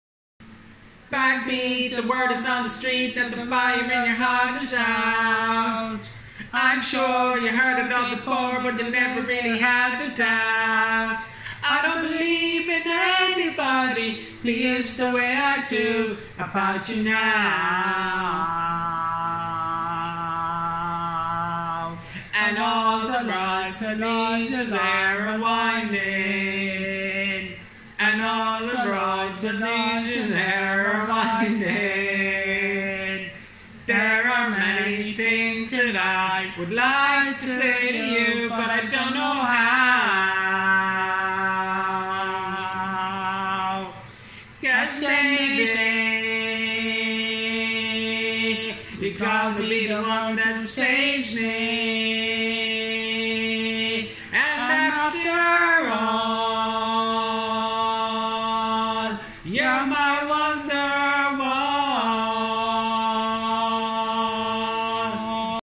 Myself and Jamie belt out a bit of Oasis
Jamie brings endless joy to me and my family. We were gifted when we were blessed with such a special boy 😊